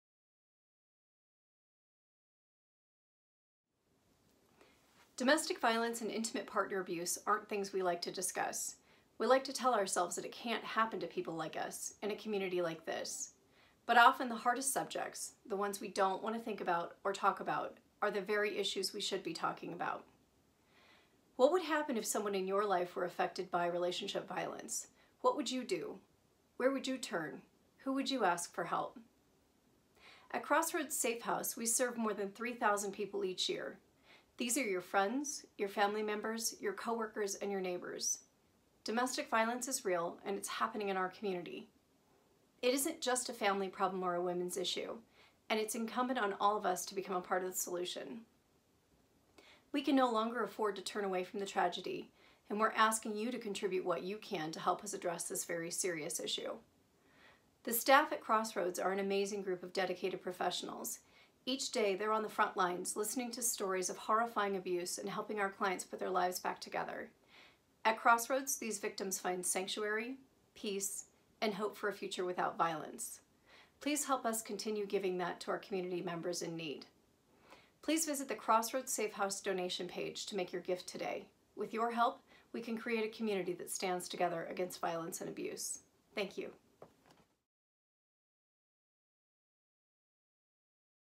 Crossroads Safehouse: January Update
Lisa Poppaw of Crossroads Safehouse shares a message about 2019 and how we can help.